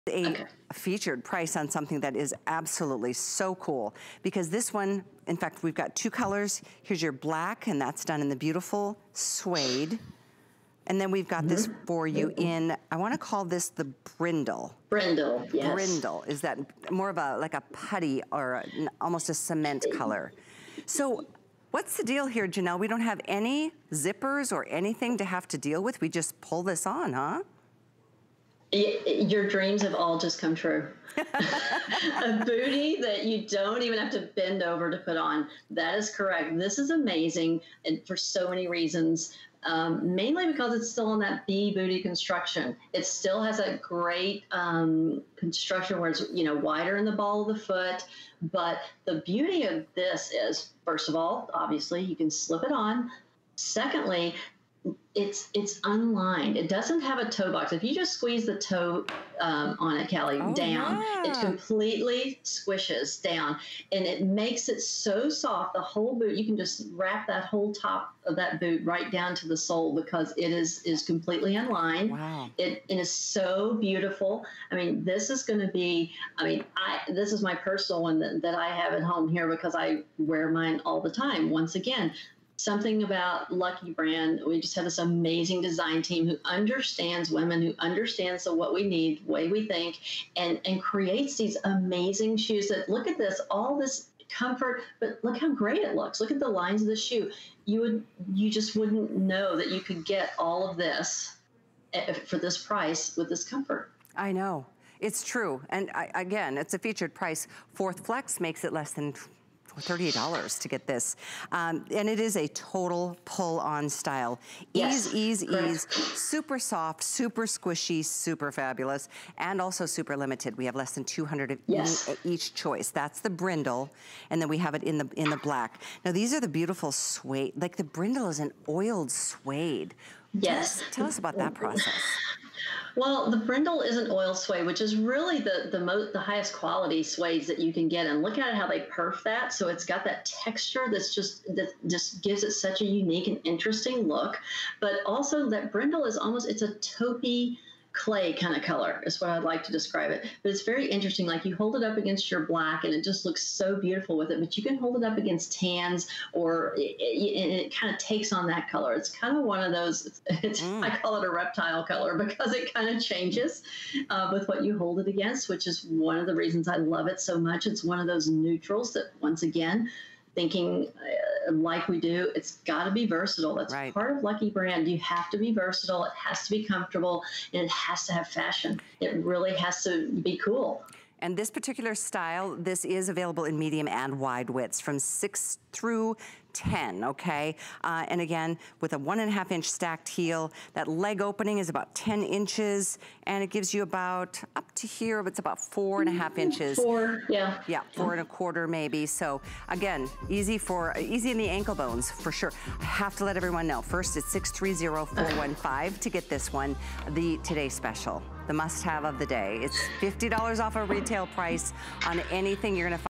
Lucky Brand Leather or Suede Baley Bootie
It's all about the shoes. You can turn any ensemble into a chic fashion statement with the simple addition of these notsosimple boots. The bootie style with an edgy perforated pattern and doubledip sides instantly ups your style status.